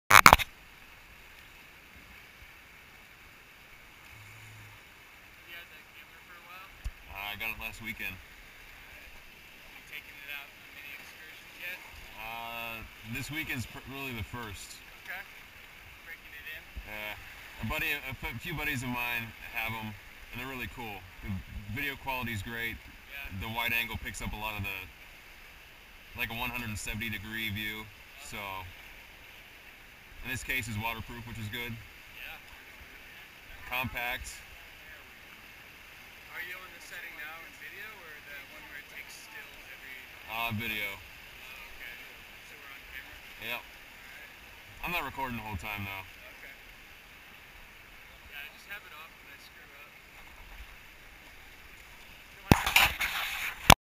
GOPR0295.MP4 - Rafting Glenwood Springs, CO - GoPro 1280 x 720 H.264 - 7.2.2011
Rafting the Colorado River in Glenwood Springs, CO

GoPro 1280 x 720 H.264
7.2.2011
- Feel free to use this video for your projects.
- Please add a mention to your video and send a link to the finished product; I'd love to see it!